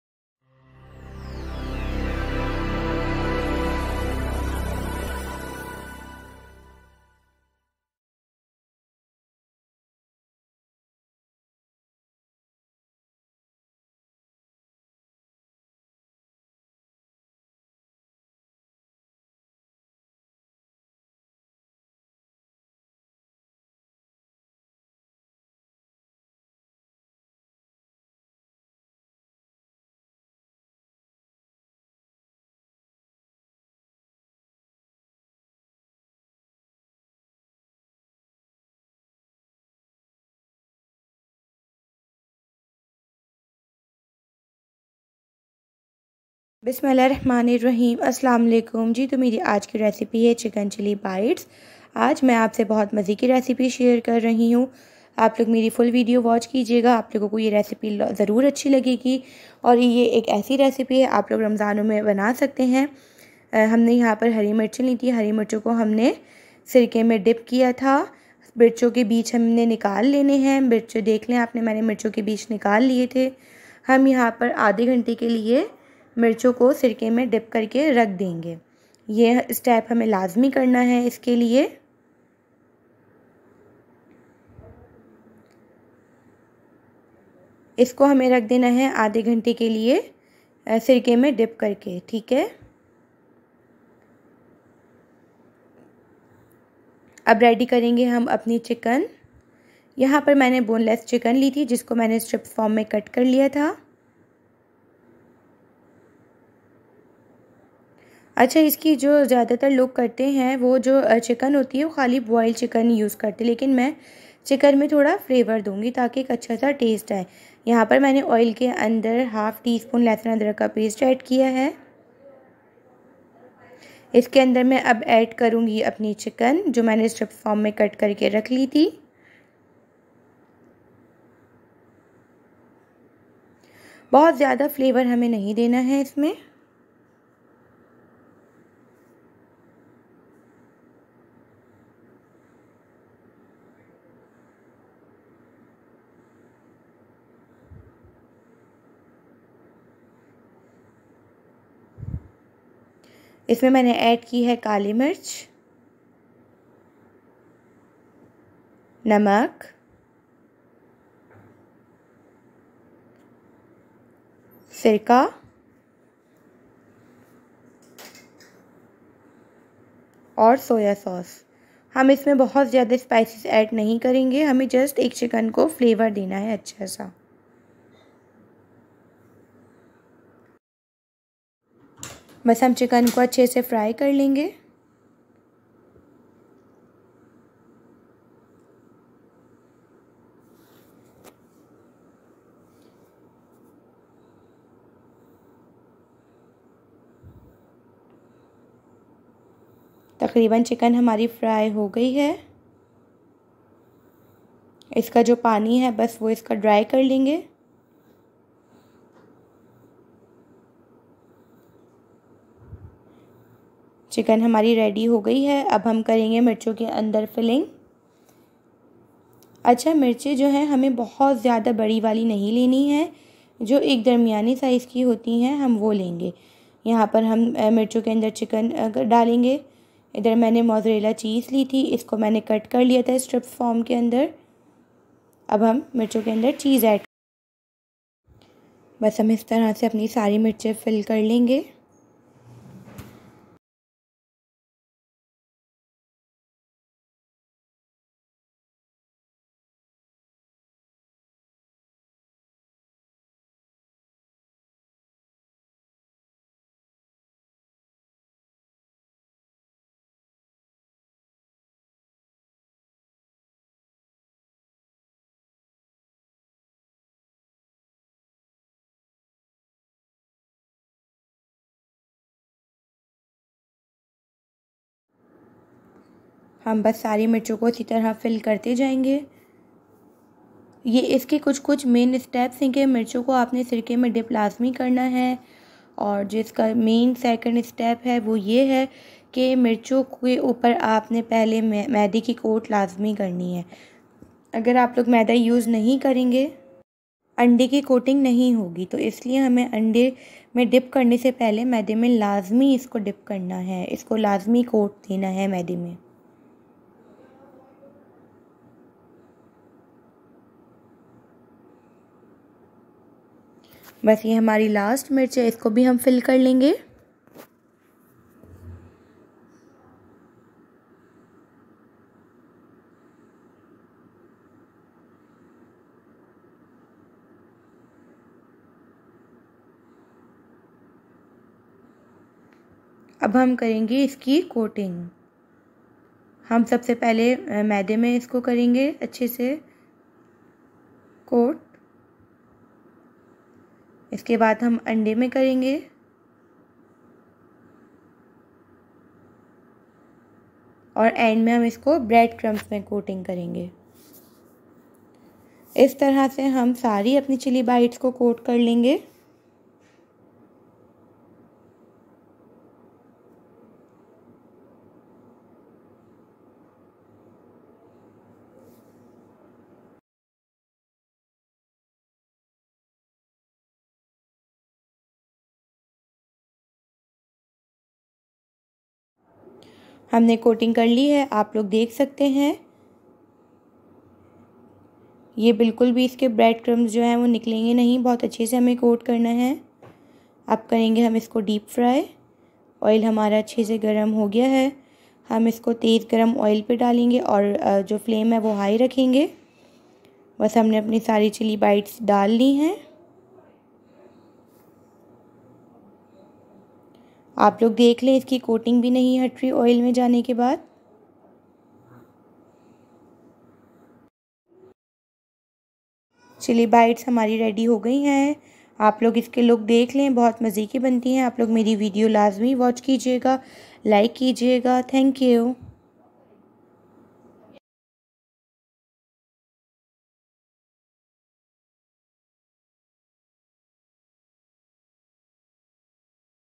Chicken Chilli Bites Recipe/ramzan special recipe by#cooking for me
Assalam o Aliakum my today recipes is chicken chilli bites go and watch my recipe